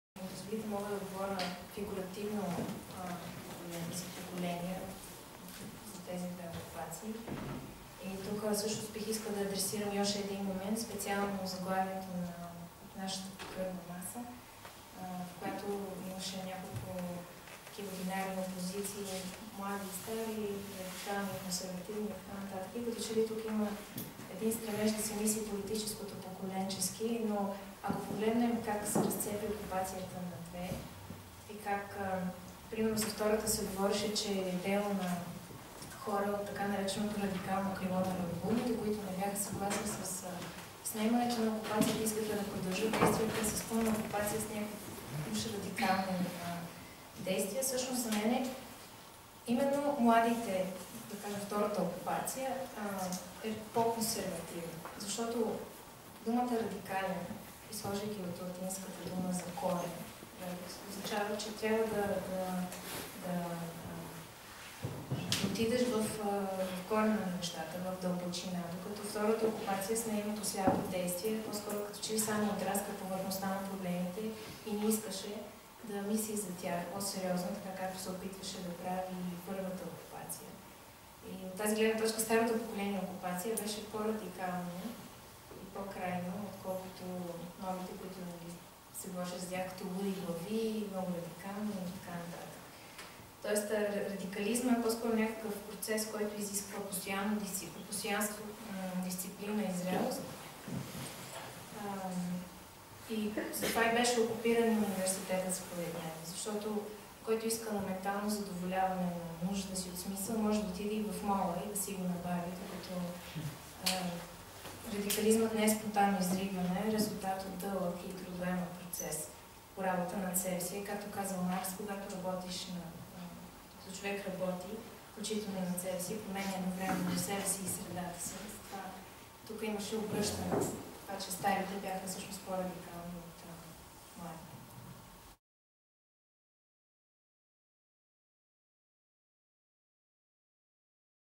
portal kultura -- Жана Цонева
Портал за култура, общество и изкуство -- Жана Цонева за новите пропасти между поколенията